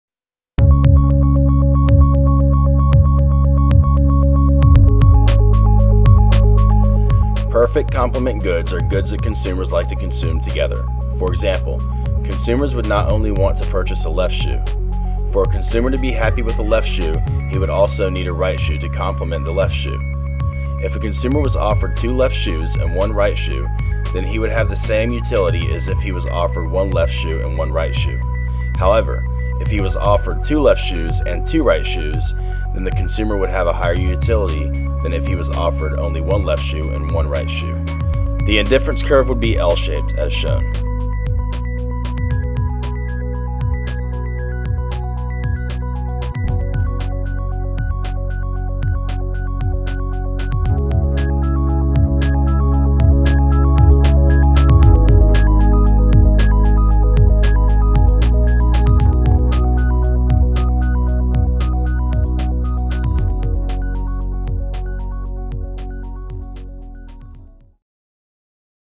Perfect Compliment Goods - DEFINITION - Indifference Curve and Utility
Perfect complement goods are goods that consumers like to consume together. For example, consumers would not only want to purchase a left shoe. For a consumer to be happy with a left shoe he would also need a right shoe to compliment the left shoe. If a consumer was offered two left shoes and one right shoe then he would have the same utility as if he was offered one left shoe and one right shoe. However, if he was offered two left shoes and two right shoes then the consumer would have a higher utility than if he was offered only one left and one right shoe. The Indifference Curve  would be L-shaped.